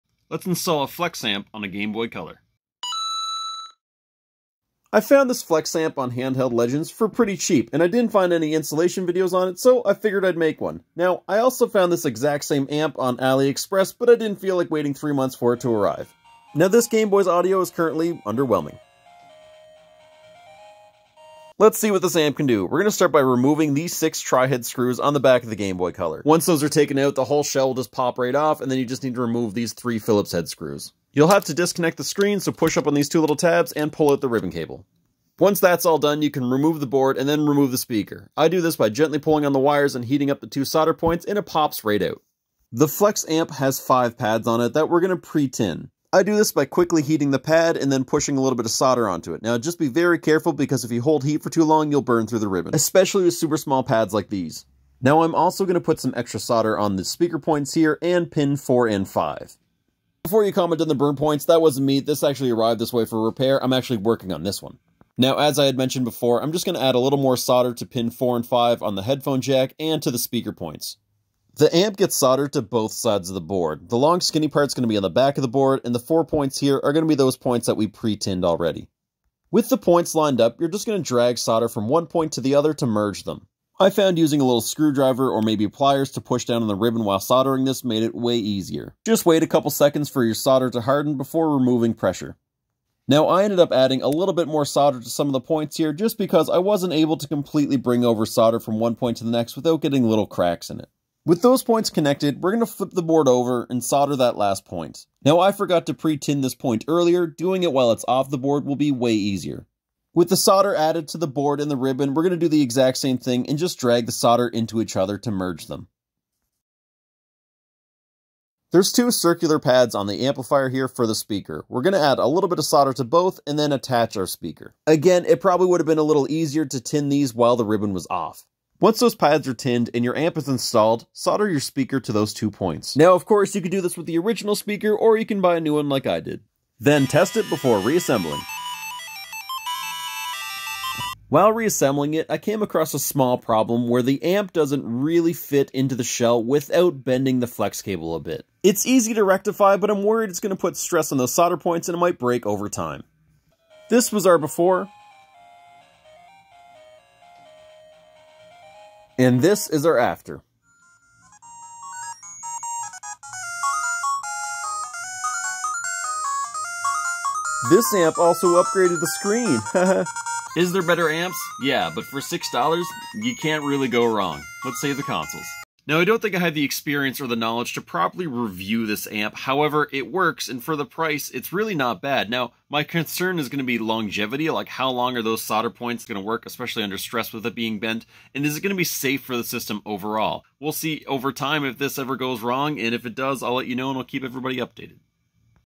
How To Install A Gameboy Color Flex Amp
A very easy installation for a very cheap amp. It's advertised to increase audio by 2-3x and I'd low power consumption.
It not fitting correctly could very well be from improper installation. Unfortunately this is my first one and I haven't seen any others examples to go by just yet!

Merch on my Website!

Want to get this amp for super cheap? here's an Ali Express link!